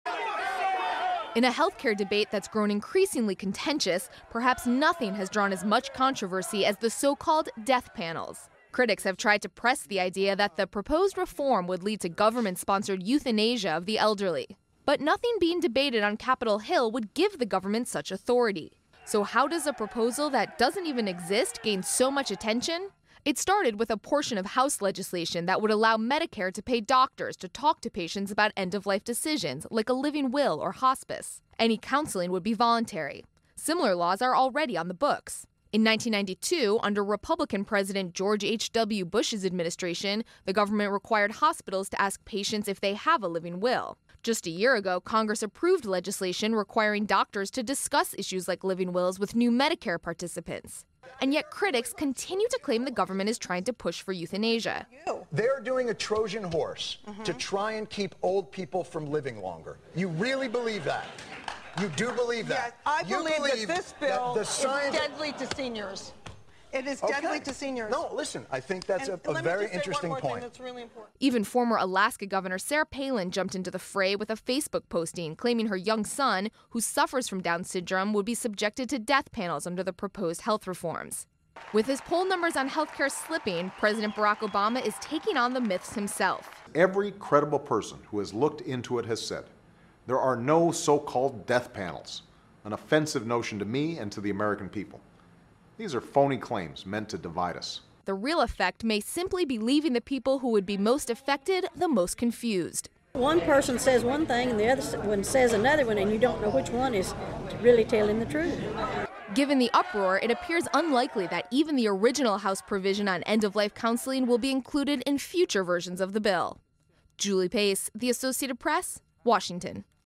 Heath Debate: Where Did 'Death Panel' Come From?
Despite the fact the idea of death panels doesn't exist in any proposed legislation, the concept has caught fire in the contentious health care debate. The AP looks at where the debate started. (Aug. 24)